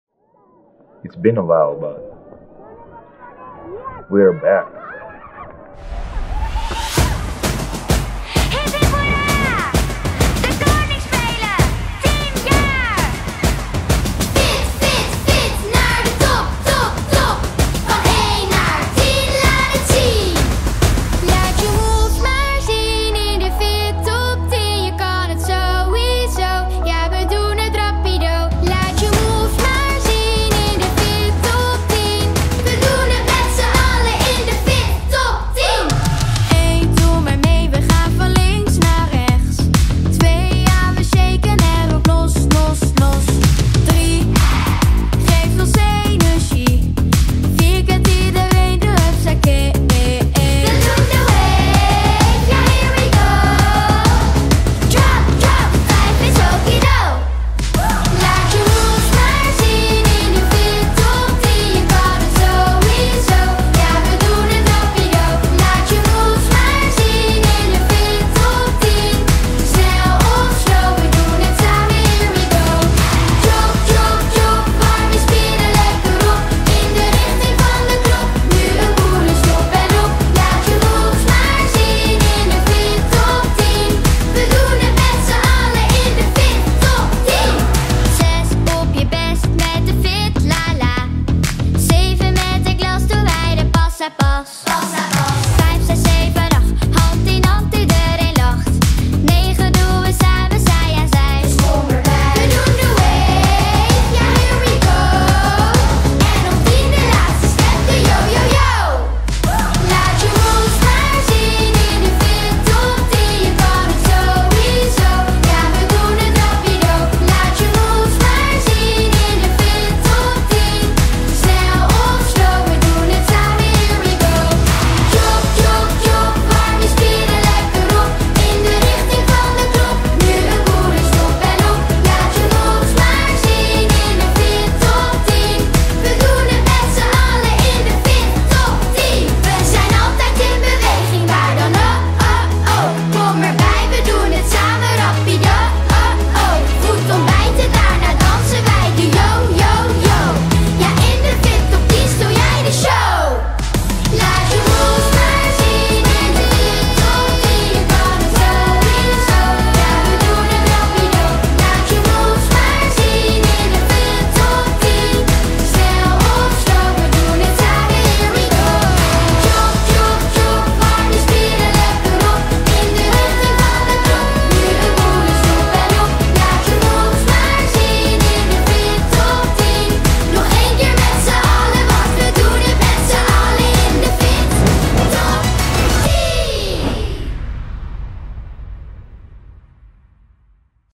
FitTop 10 || Kudawecha Aruba: Koningsspelen 2022
On Aruba we celebrate Kings day. And together with my ex school Scol Primario Kudawecha the celebration is back ! They picked up the celebration in typical style and I had the pleasure to capture this ones again. Lots of dancing, fun, games and lots of water. Song: copyright of kinderen voor kinderen. Video : povitree/Levy Geerman CREATE | TEACH | INSPIRE